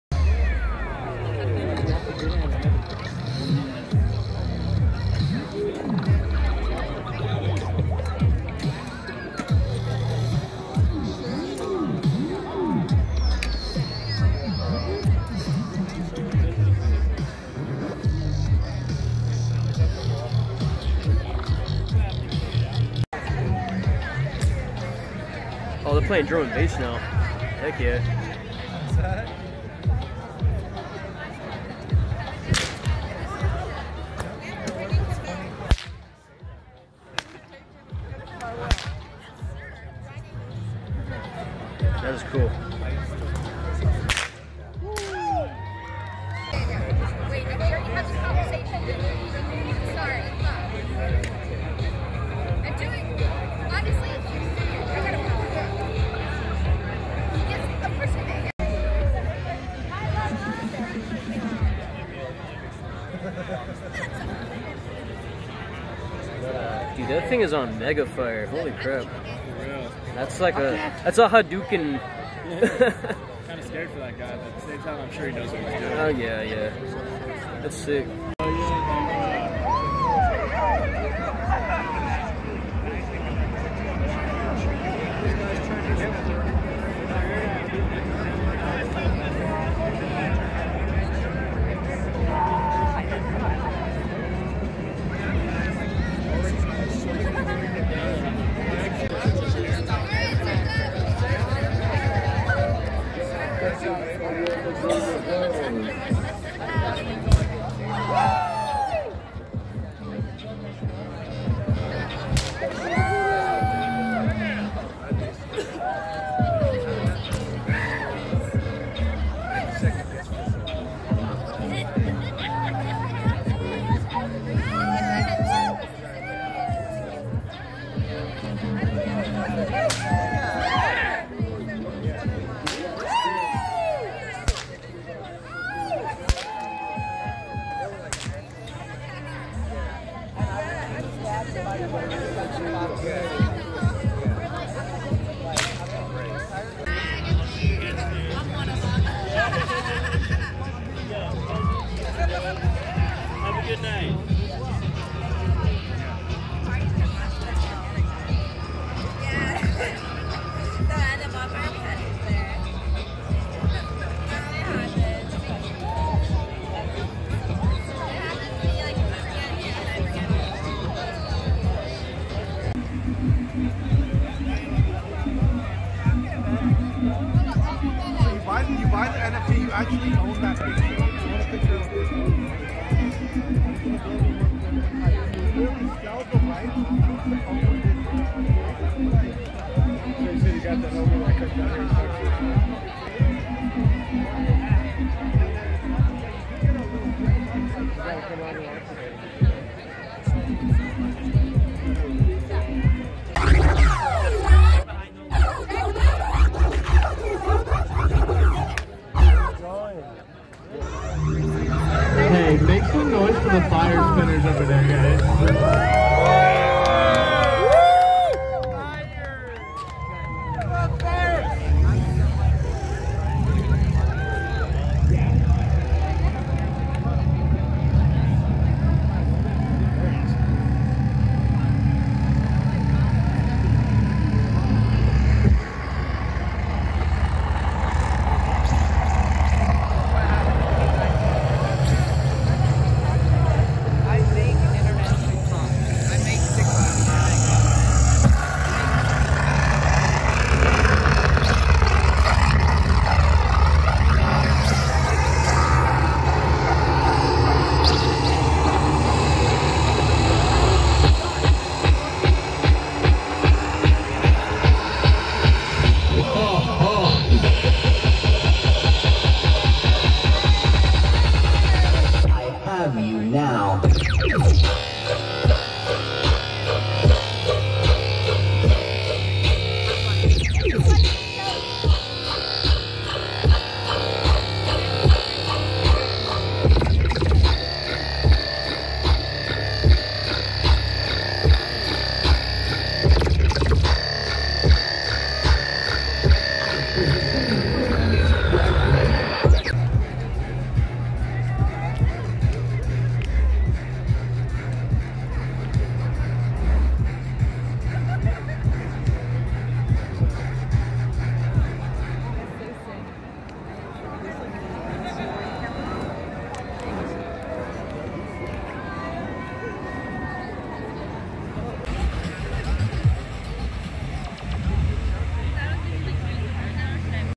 Wednesday Flow Group - Ocean Beach CA 02-16-2022
Fire spinners and poi gathering at Ocean Beach, San Diego California.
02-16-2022